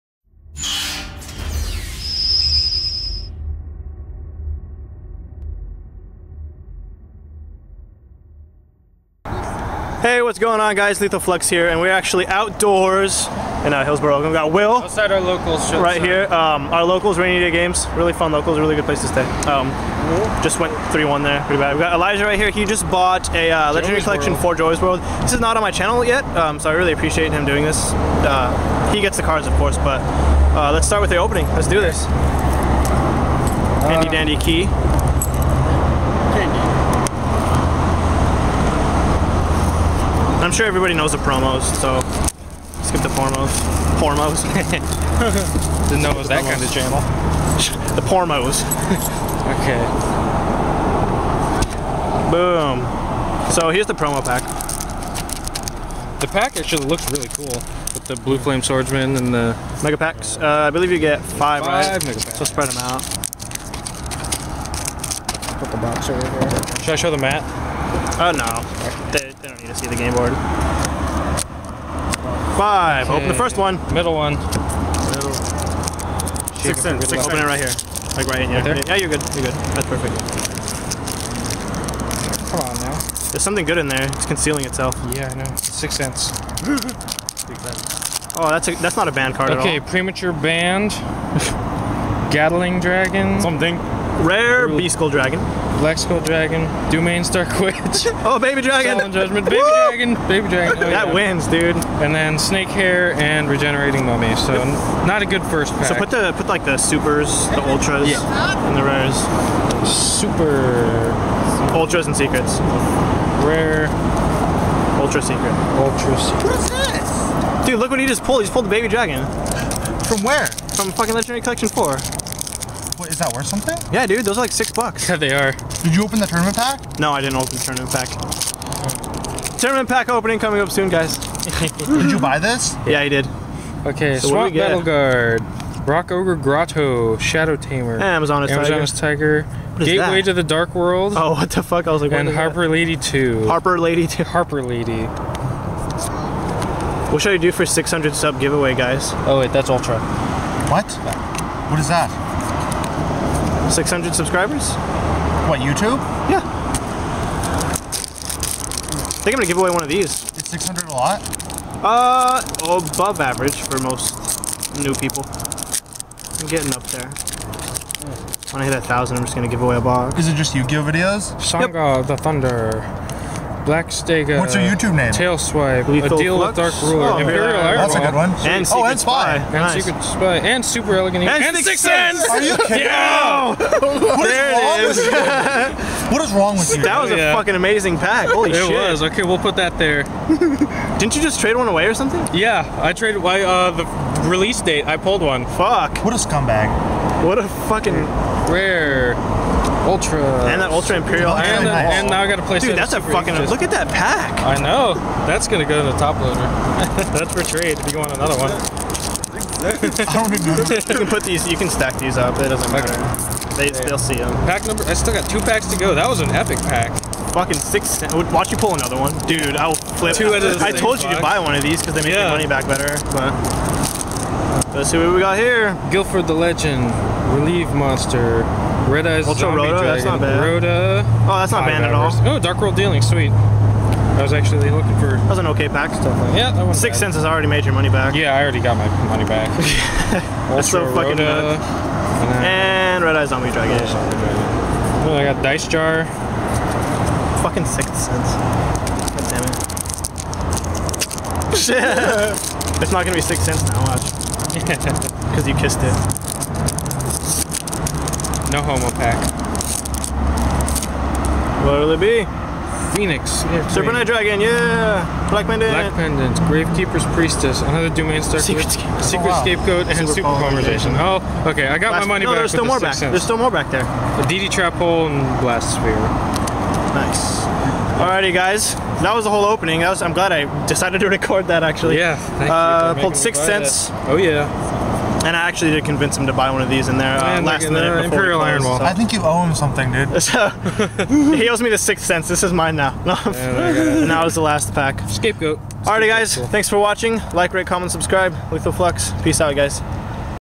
Legendary Collection 4 Opening!!
Hey guys, it's been i while since i have put up any pack openings. So here is a legendary collection 4!! PEACE OUT!